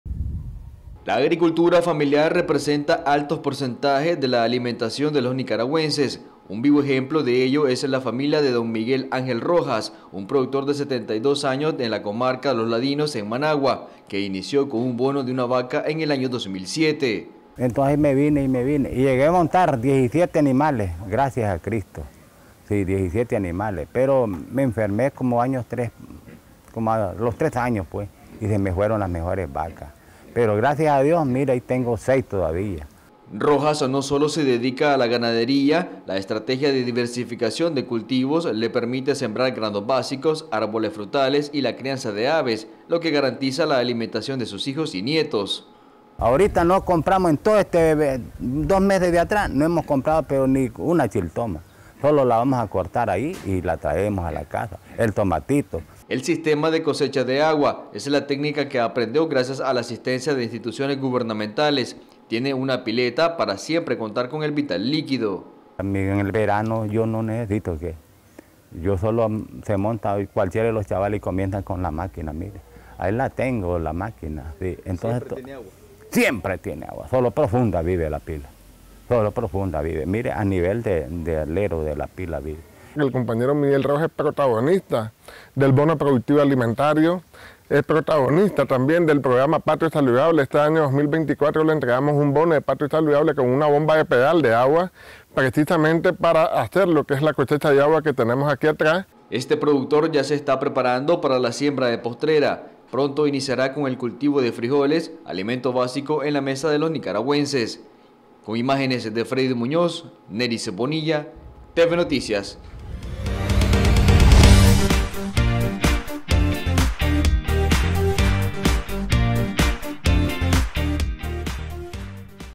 Agricultura familiar diversifica los cultivos y garantiza la seguridad alimentaria en el país
La agricultura familiar representa altos porcentajes de la alimentación de los nicaragüenses, un vivo ejemplo de ello es la familia de don Miguel Angel Rojas, un productor de 72 años de la comarca Los Ladinos en Managua, que inició con un bono de una vaca en el año 2007.